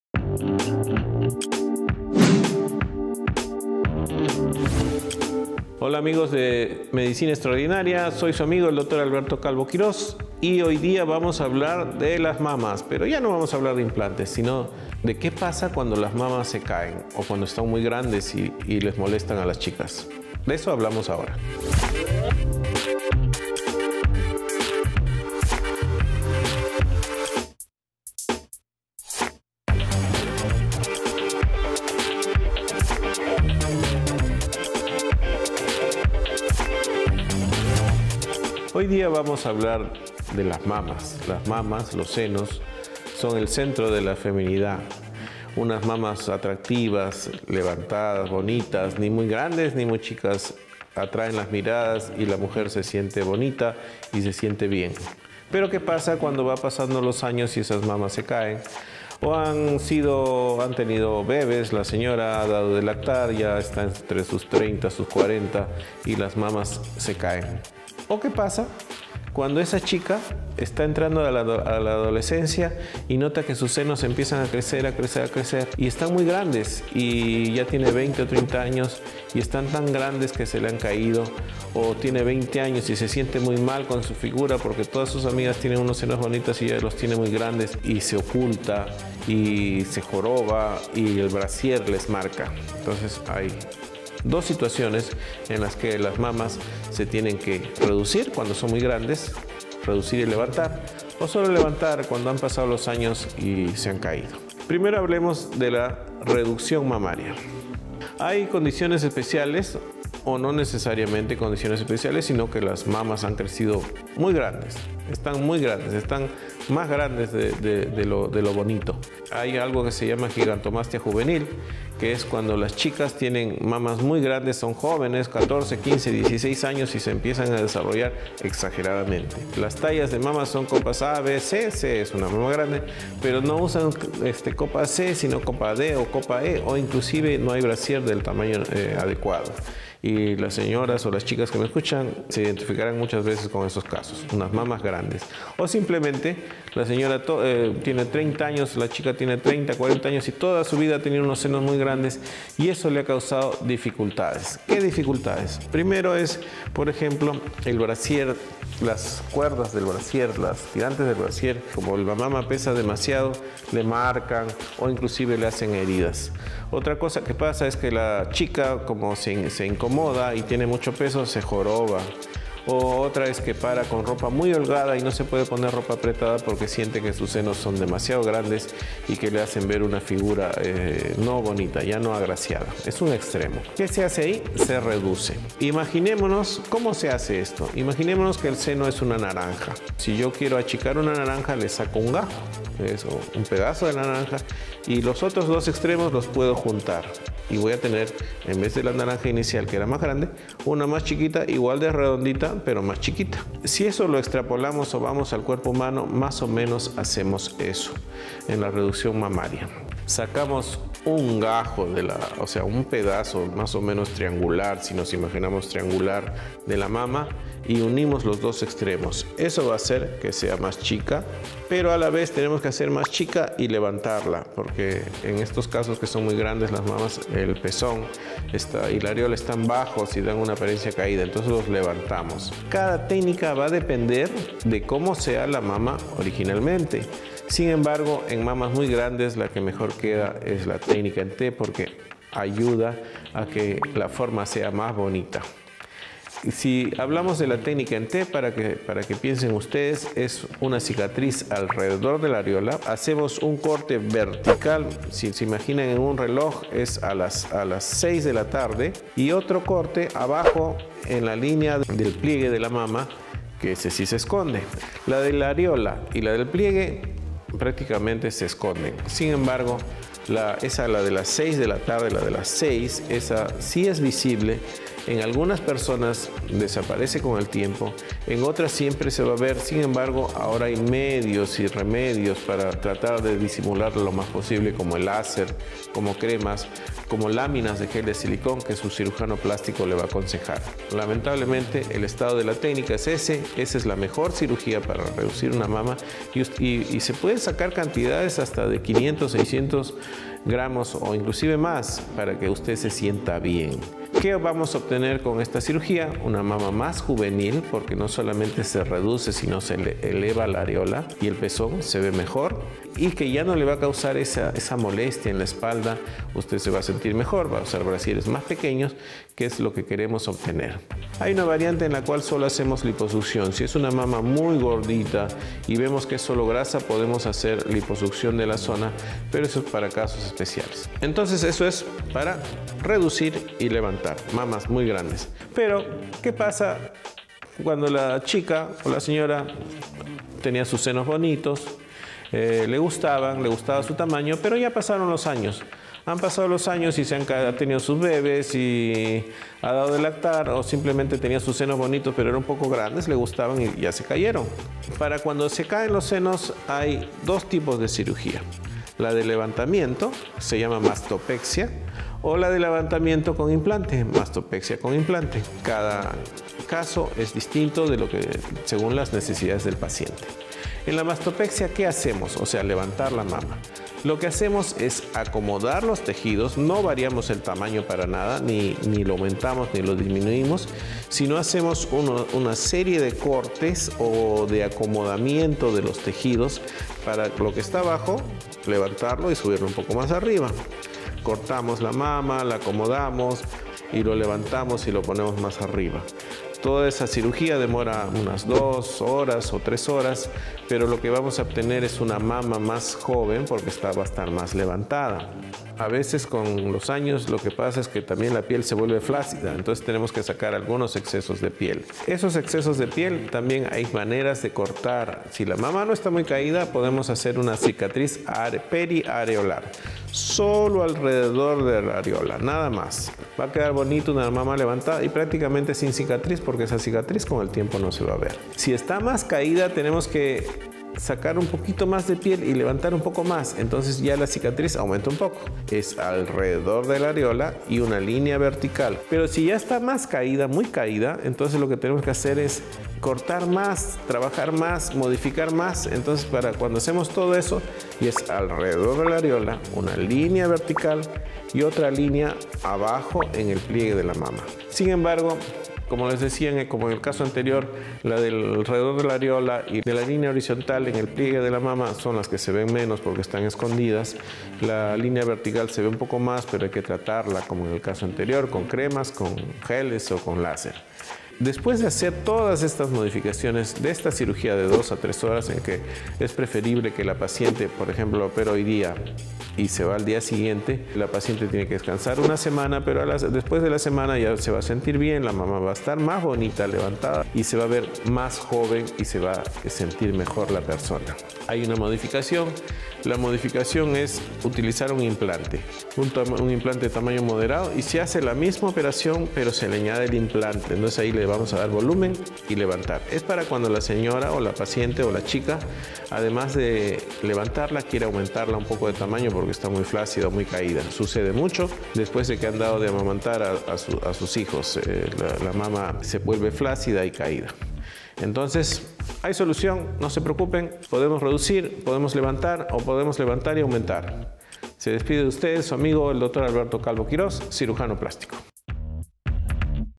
SENOS CAIDOS 👙 LEVANTAMIENTO y REDUCCIÓN de MAMAS 👙 MASTOPEXIA con y sin IMPLANTE 🌹 Dr ALBERTO CALVO
💛 MASTOPEXIA 🧡
Los senos son una de las partes más llamativas y atractivas de las mujeres y tener unos senos hermosos puede atraer muchas miradas. Cuando las mamas crecen demasiado o simplemente caen por el paso de los años o por la gravedad, los senos pueden ser un verdadero problema, pero esto tiene una solución rápida a través de la cirugía plástica. El Dr Alberto Calvo Quiroz nos lo explica.

📌 Av. Arequipa 1148 - 7mo. piso Consultorio 701. Lima-Perú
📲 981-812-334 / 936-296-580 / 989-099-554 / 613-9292
📲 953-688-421 (Para pacientes del extranjero)